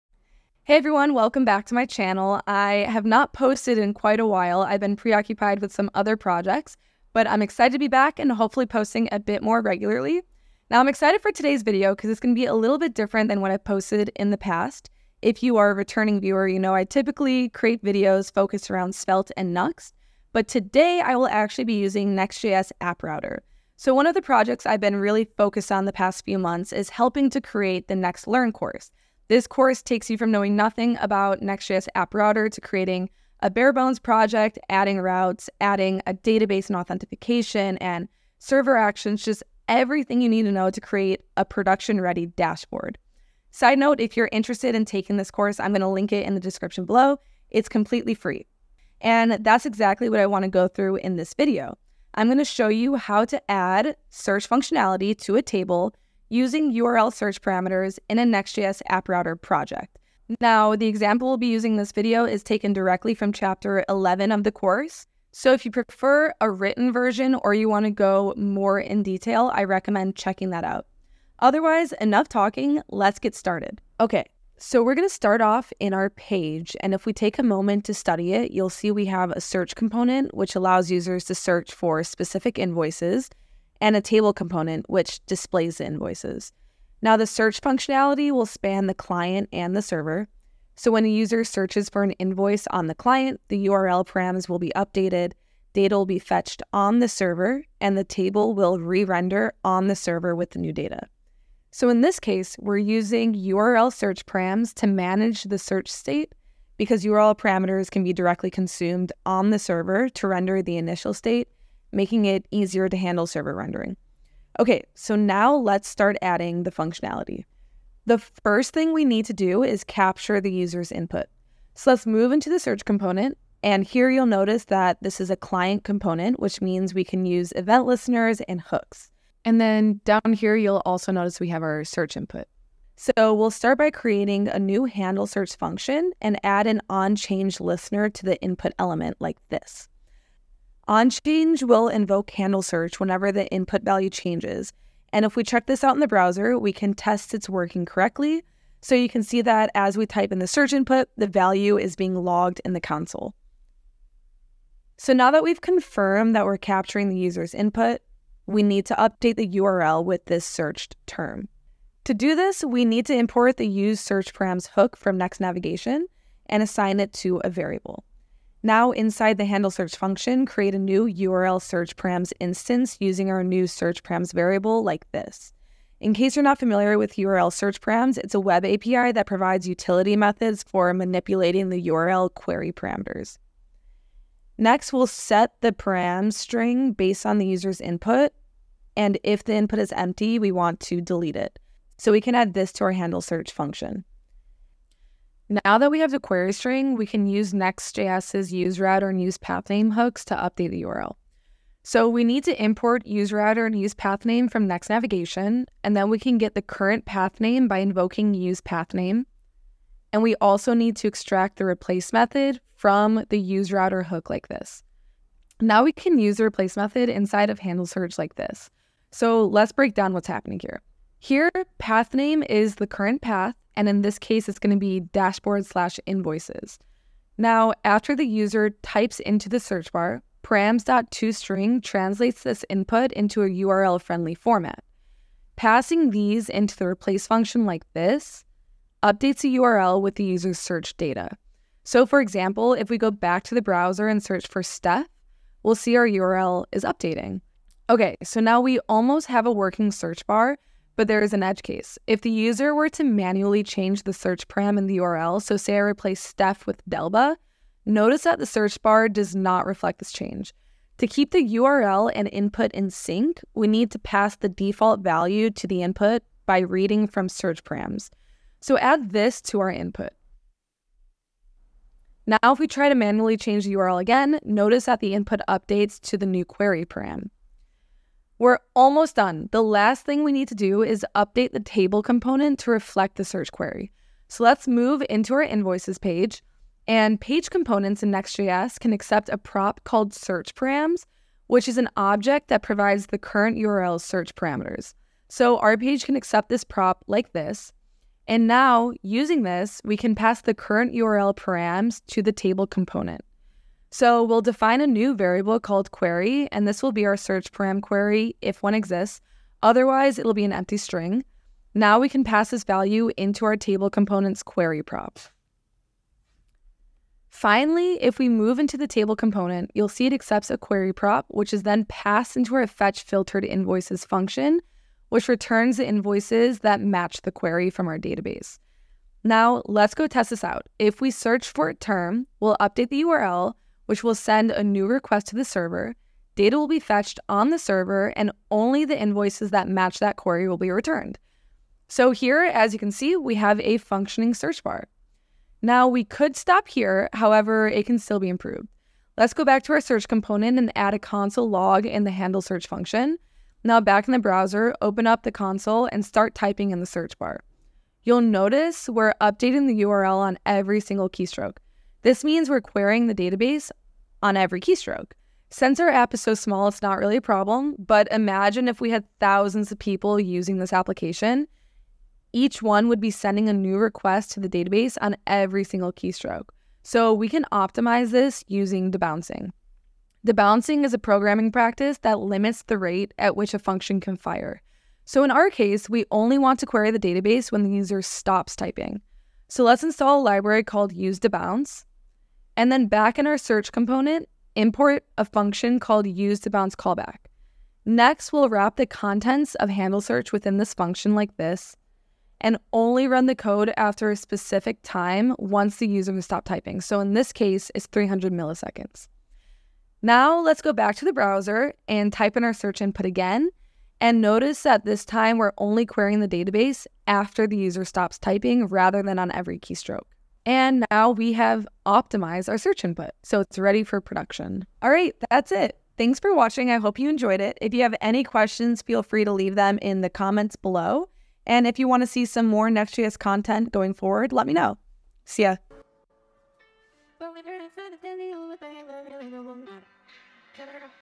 Adding a Search Feature to a Server Component | Next.js App Router
Learn how to use URL search parameters to add search functionality to a server component in a Next.js app router project.

0:00 - Intro
1:13 - Overview of page code
2:01 - Capturing the user's input
2:41 - Update URL with search param
4:13 - Keeping the url and input in sync
4:45 - Updating the table based on search query
5:42 - Debouncing